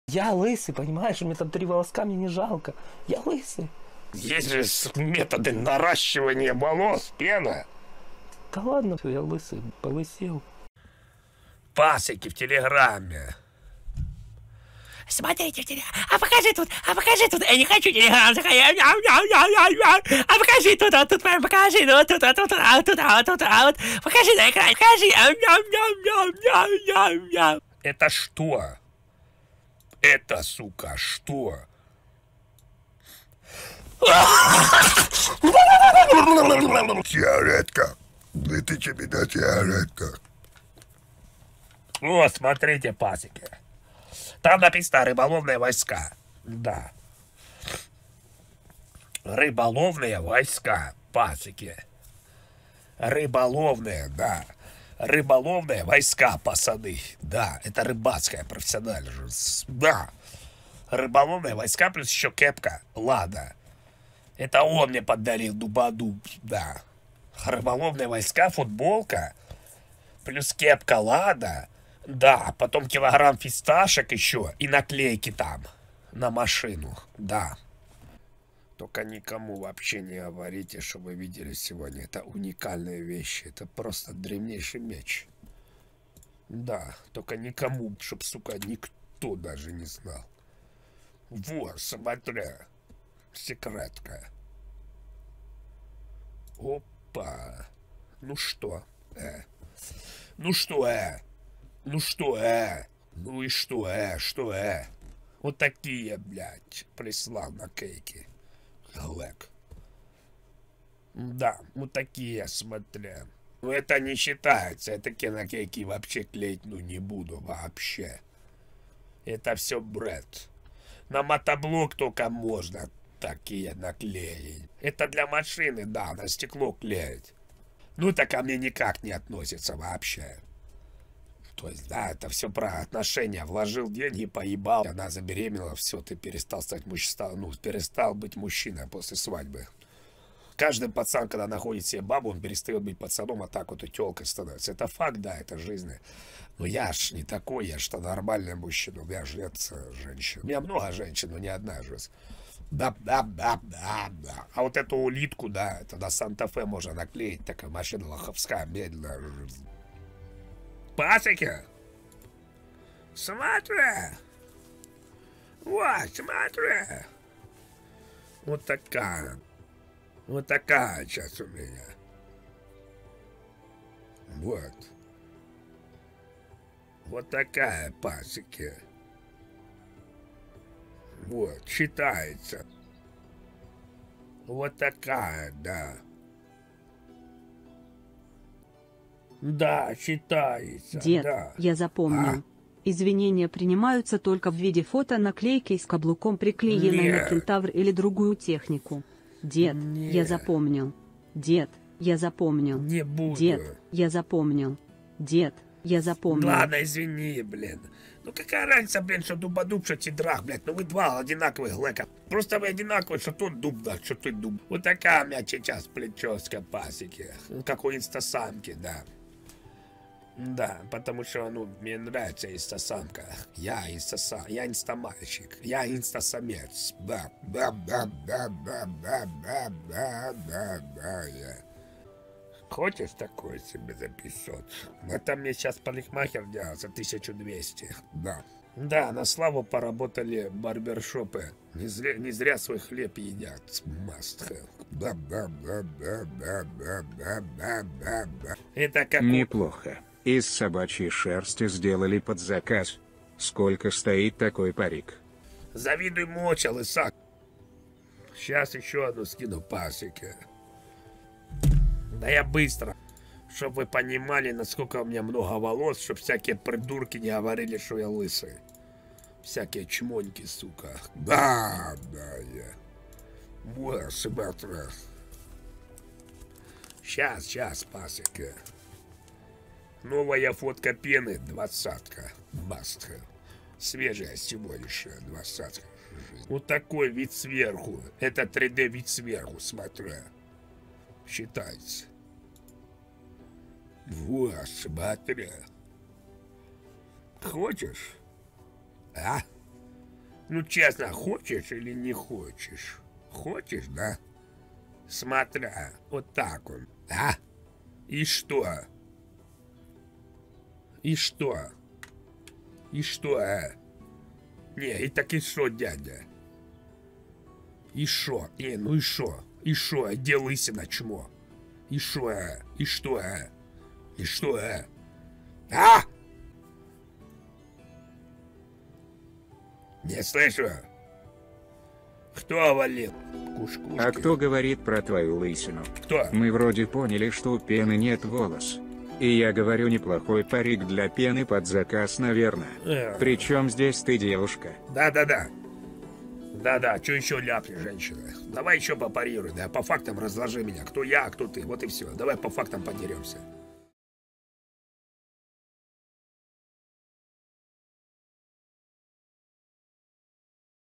Валакас доказывает что он не ЛЫСЫЙ и показывает ШМОТ
Дисклеймер:
00:00 Начало
00:14 Ням ням ням
00:29 Шмот рыбака
01:32 Наклейки
02:40 Причёска
04:45 Причёска 2